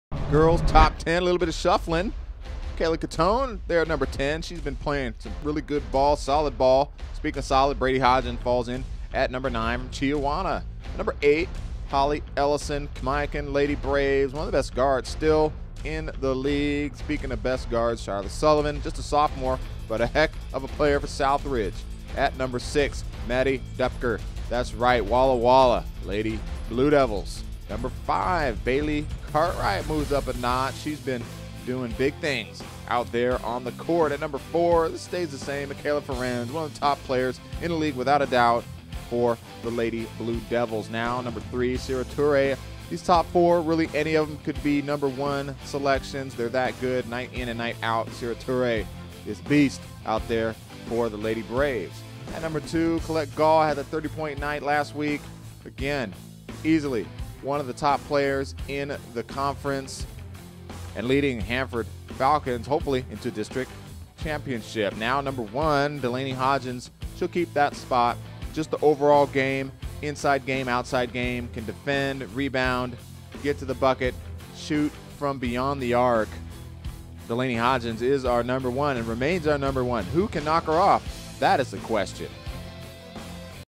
Mid Columbia Girls Top 10 - 1/30/14
Top 10 players as selected by washington sportsnetwork.com heading into 1/30/14 games